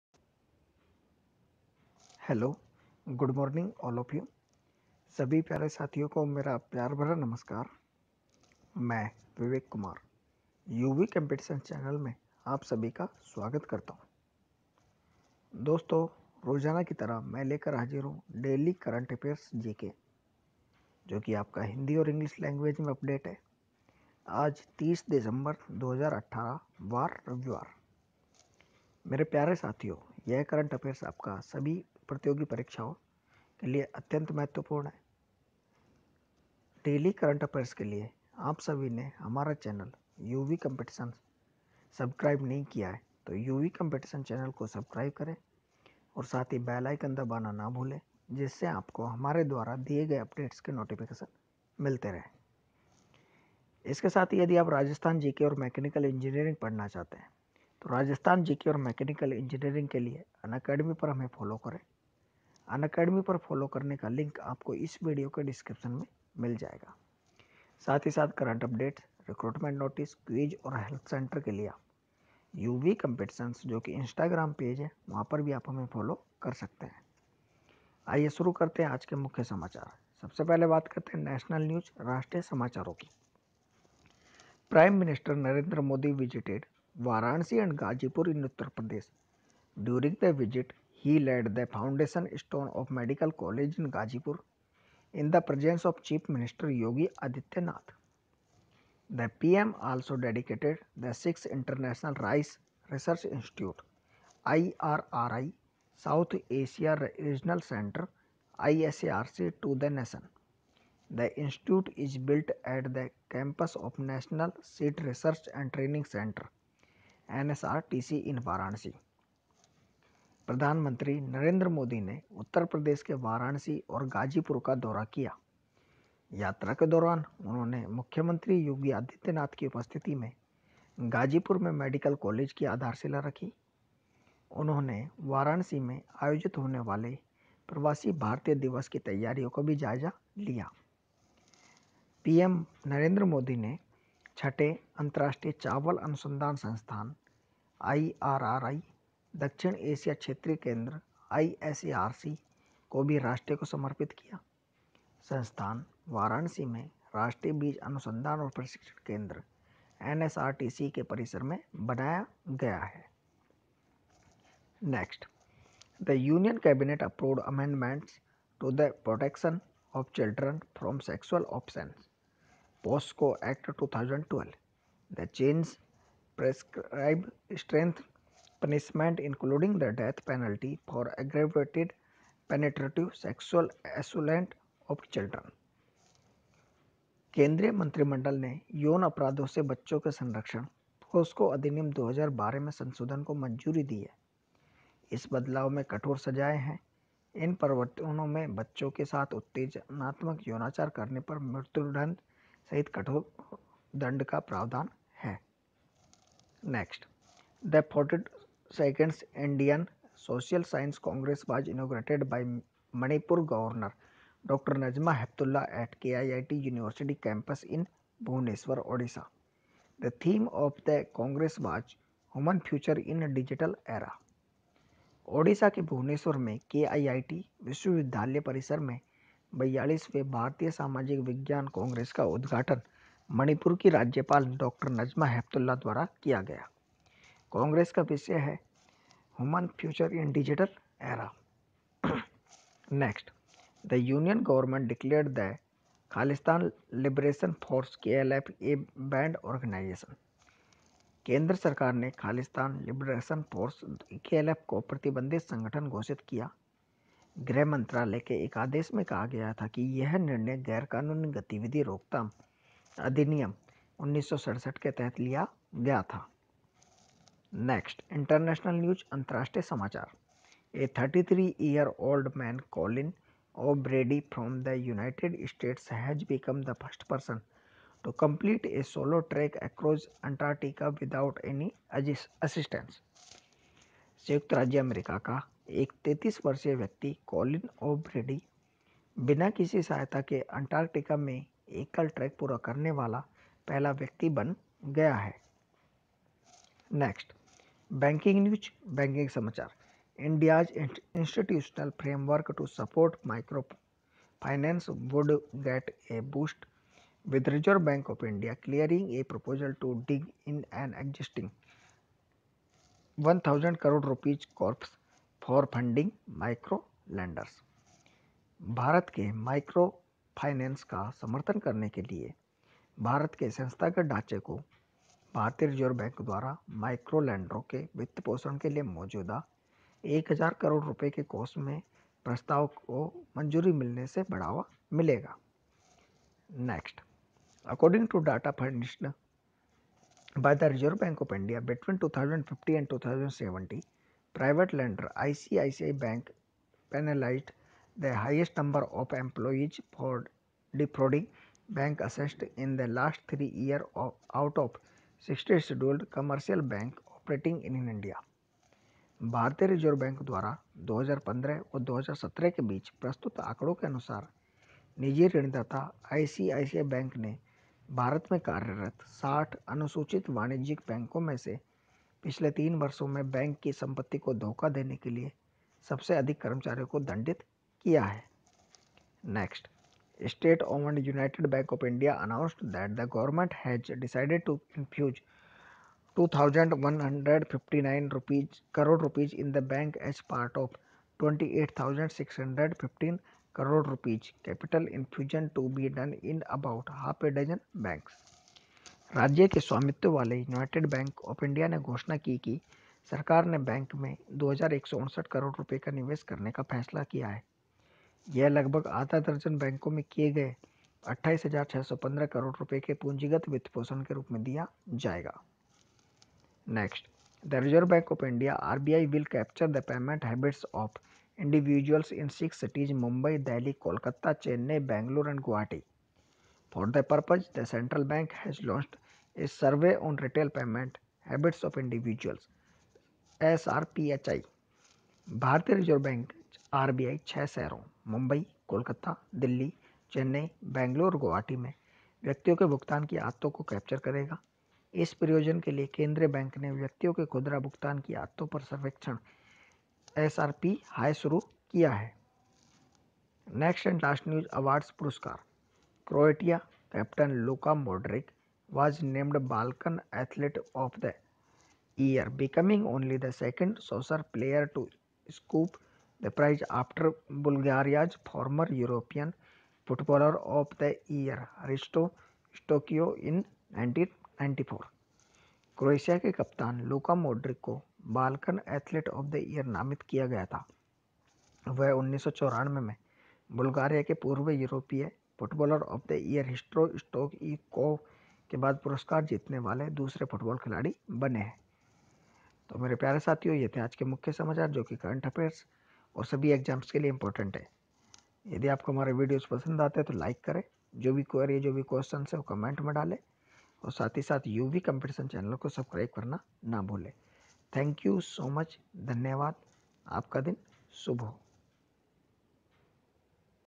Daily Current Affairs GK updates 30 December 2018 in Hindi & English for UPSC, SSC, Banks Exams
Daily Morning Current Affairs GK updates 30 December 2018. On our YouTube Channel

Downlaod PDF -